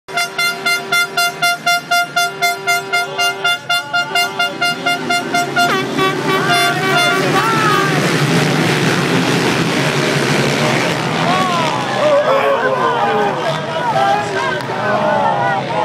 2011_09_25_nazioni_rigamonti.MOV
2011_09_25 Rip[resa della partenza di gara 1 del trofeo delle nazioni 2011, riprese e diritti di Roberto Rigamonti (Rana Verde) - video articolo con diretta evento e interviste a nicola Montalbini ed Emanuele Giovanelli si quaddisti.it - Radio Box.